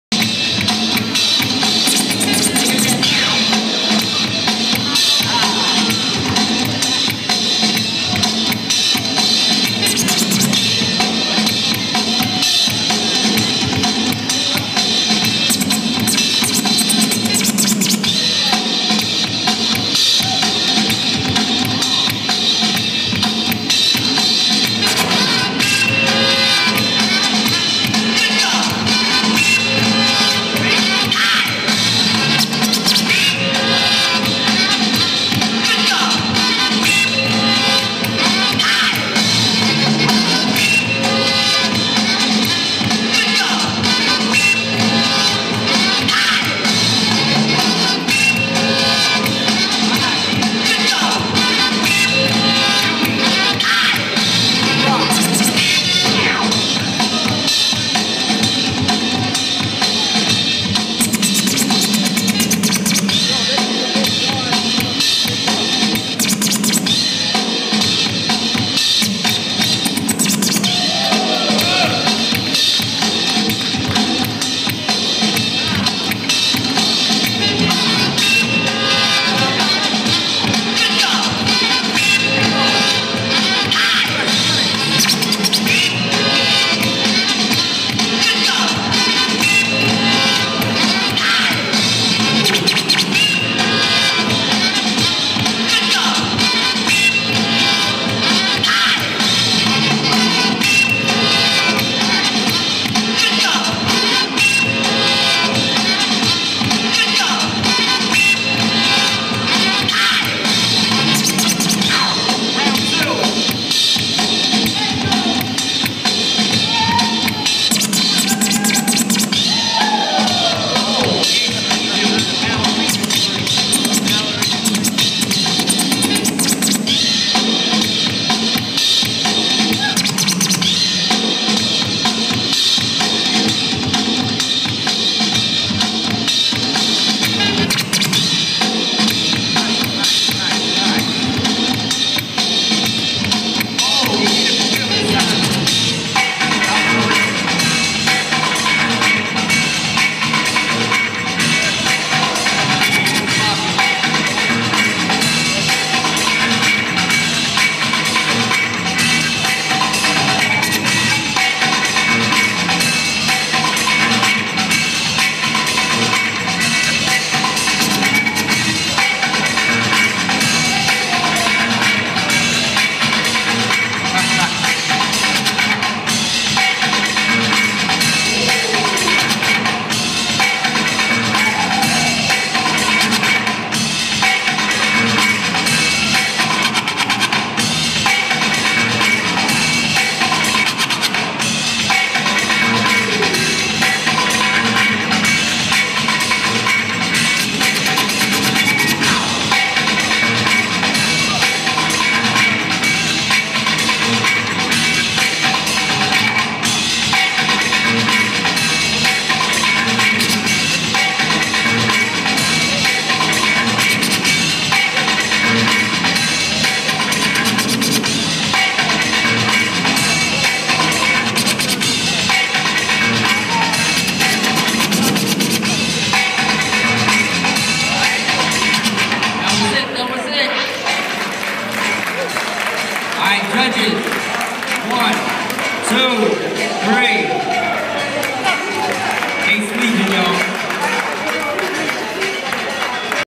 International BBoy/BGirl Games 2012
Competitors in the International BBoy/BGirl Games 2 on 2 Battle held in Fall River MA.